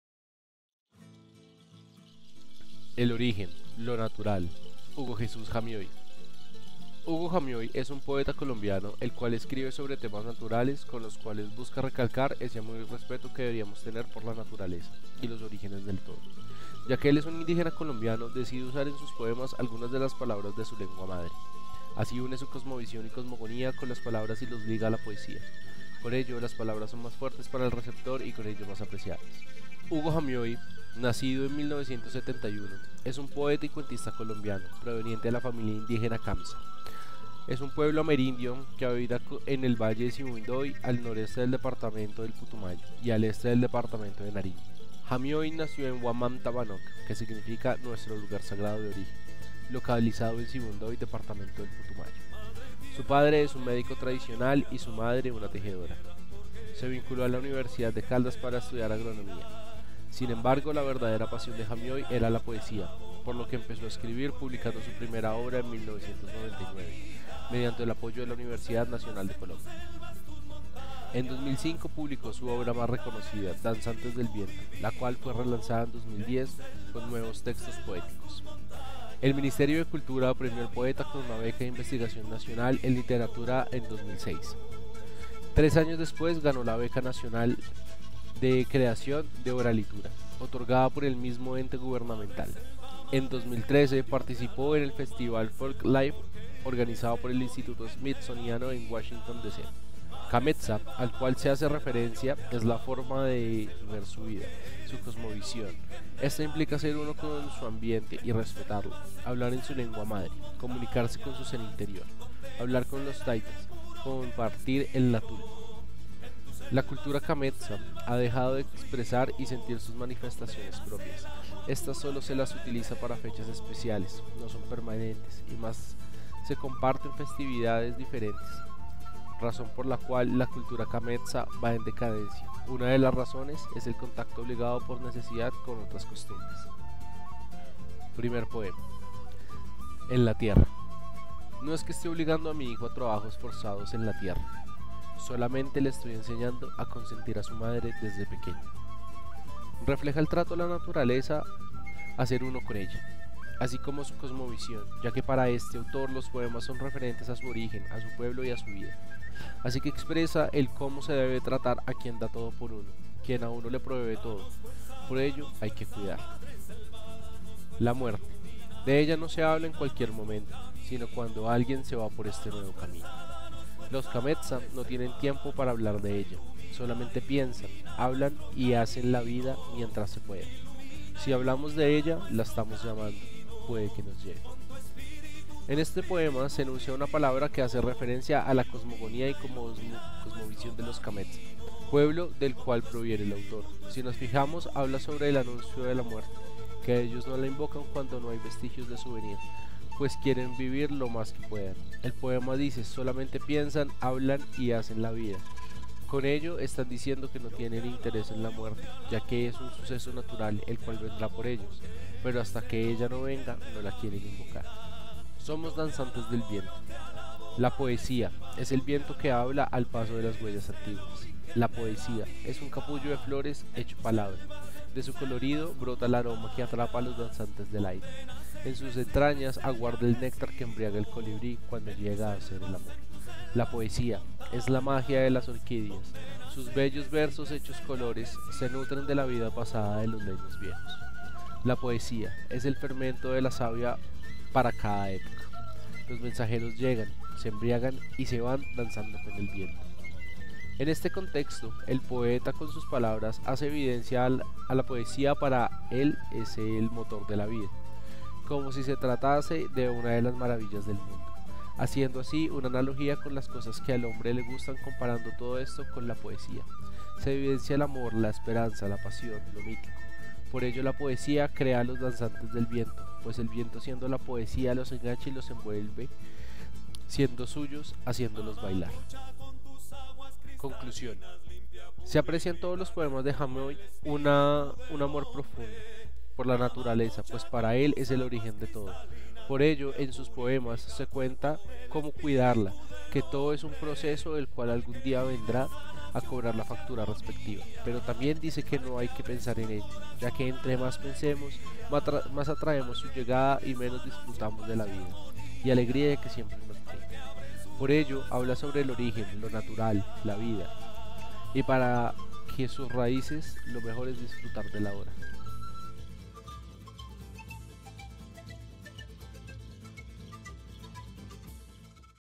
El origen, lo natural: Hugo Jesús Jamioy
Literatura Colombiana Actual
Seminario de Pensamiento Crítico

Profesor titular:
Bogdan Piotrowski

Monitor:
Juan Sebastián Nieto

Autor:
Santiago Nicolás Linares Mondragón

2020

Bibliografía

• Selección de poemas – Hugo Jamioy Juagibioy. (2018).
• Hugo Jamioy Juagibioy. (2020).
• Hugo Jamioy Juagibioy. (2013).
• ONIC. (2020). [Image].
• GOV. (2020). (Image).
• Diàsporas. (2020). (Image).
• Panoramas. (2020). Palabra de los bordes que transita a través: la oralitura como posible apertura político-cultural [Image].
• Imagenes tomadas de Pixabay.com
• Imágenes tomadas de Flickr.com
• Bartelsman, F. (2018). De agua, viento y verdor [Image].
• Prosperidad social. (2016). [Image].

• ATL Innovacion. (2019). [Image].

• Artesanías de Colombia. (2017). Carnaval del Perdón [Image].
• Sochor, J. (2013). Kamentsa shaman [Image].